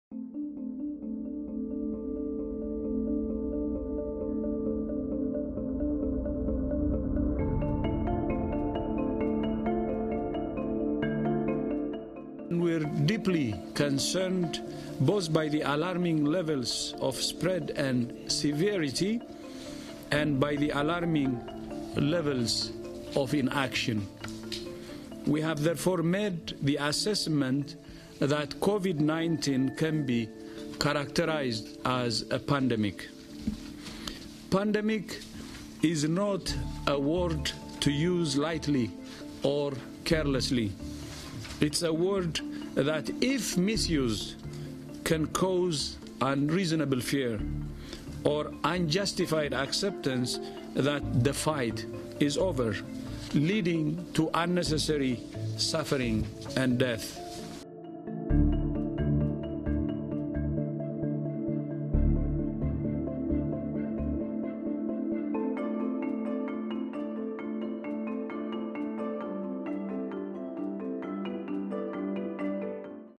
WHO declare COVID-19 a Pandemic
COVID-19 UPDATE March 11th, 2020: COVID-19 is now declared a pandemic by World Health Organisation (WHO).